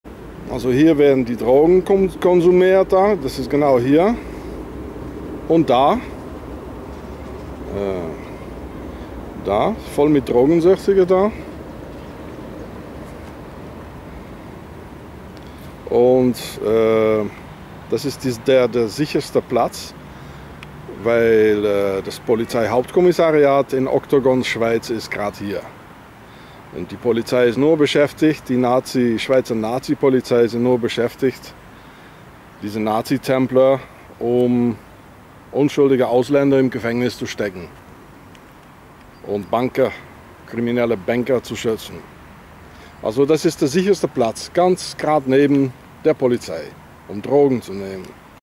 Der sicherste Platz für Drogenkonsum in der Schweiz ist grad neben dem Polizeihauptkommisariat
IN DER SCHWEIZ HABEN DROGENKONSUMENTE NICHTS VON DER POLIZEI ZU BEFÜRCHTEN; DROGEN UND DROGENSÜCHTIGE WERDEN DURCH DIE MÄCHTE DES OCTOGON GESCHÜTZT.